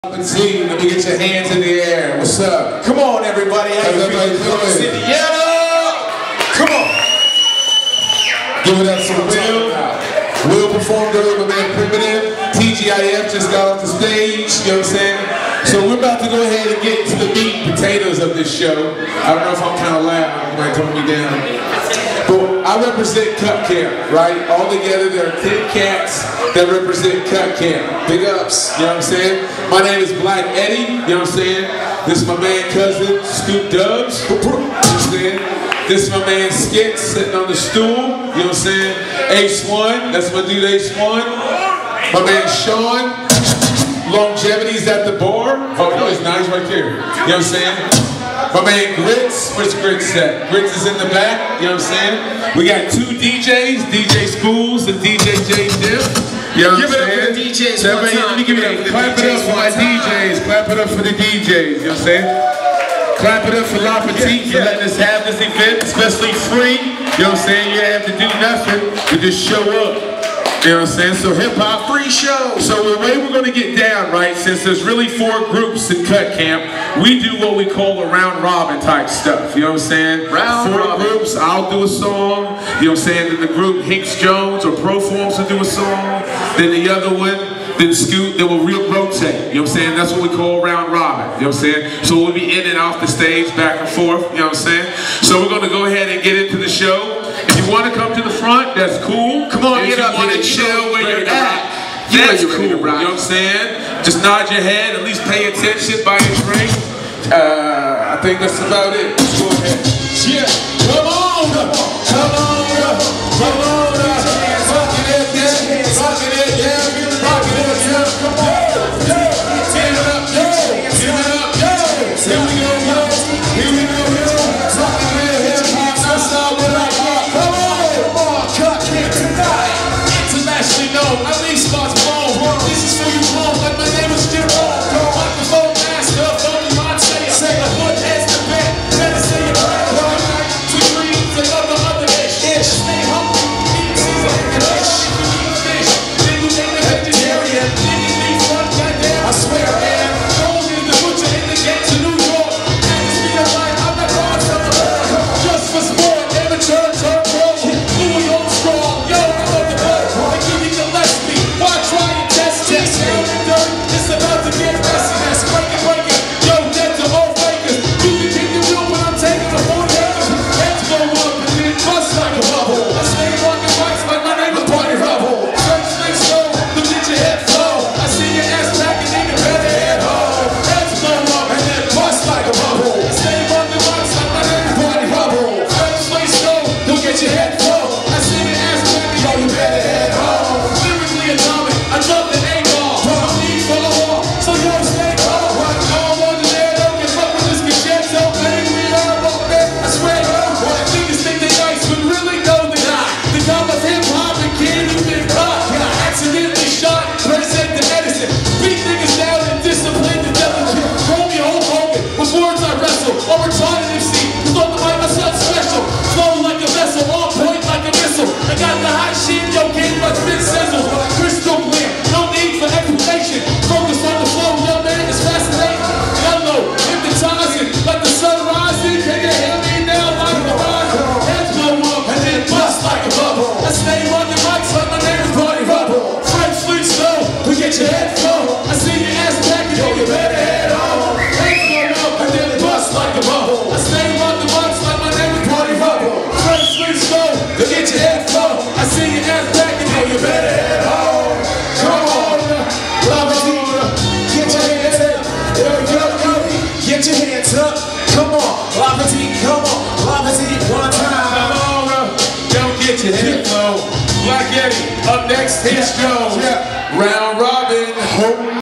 Black Eddie and Scoot Dubbs - Cut Camp @ Le Petit Caraibes 2-10-2017 part 5
Black Eddie and Scoot Dubbs perform with Cut Camp at Le Petit Caraibes in Columbus, Indiana on February 10, 2017

Check out concert and event reviews by Melodie in READ the Music